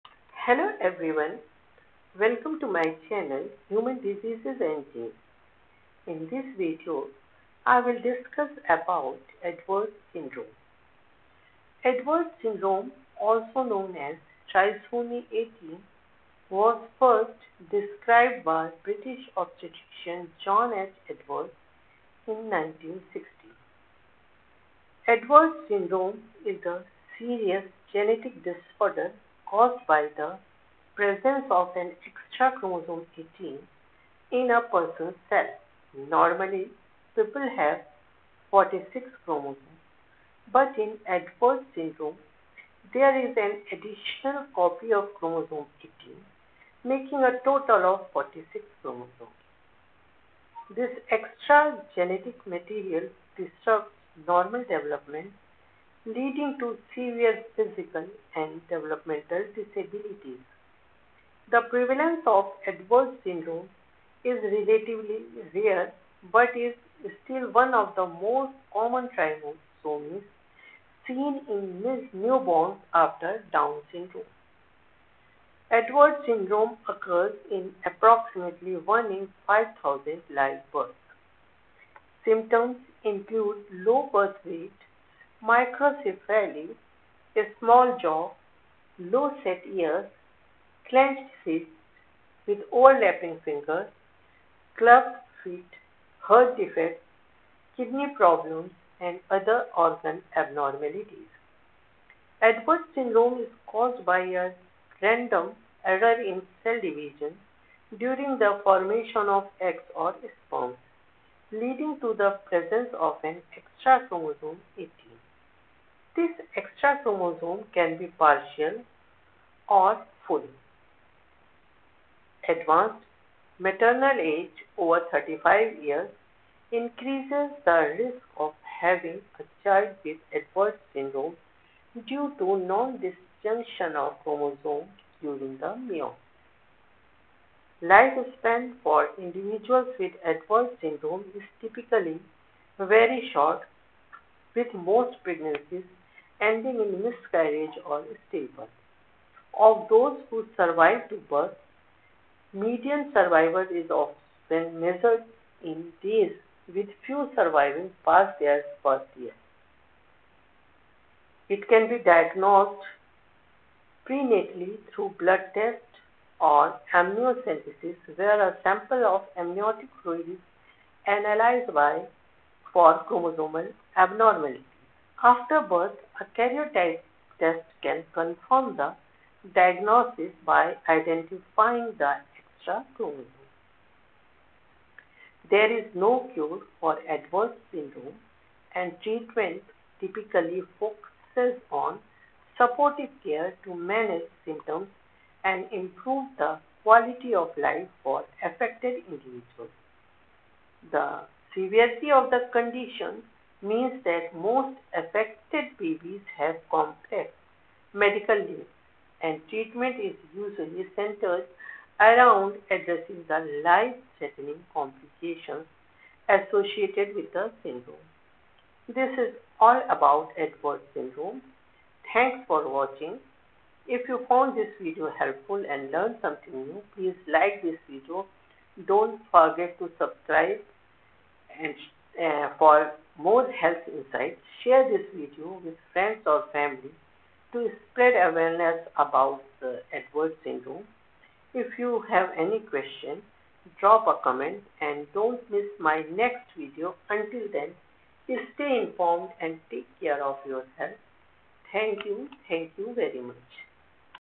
What is Edward's syndrome?
Hello viewers,
Welcome to my channelLectures on Human diseases and genes.

Edward's syndrome, also known as trisomy 18, was first described by British obstetrician John H. Edwards in 1960. Edwards syndrome (also known as trisomy 18) is a serious genetic disorder caused by the presence of an extra chromosome 18 in a person's cells. Normally, people have 46 chromosomes (23 pairs), but in Edwards syndrome, there is an additional copy of chromosome 18, making a total of 47 chromosomes. This extra genetic material disrupts normal development, leading to severe physical and developmental disabilities. Edwards syndrome occurs in approximately 1 in 5,000 live births. Symptoms include low birth weight, microcephaly, small jaw, low-set ears, clenched fists with overlapping fingers, club feet, heart defects, kidney problems, and other organ abnormalities.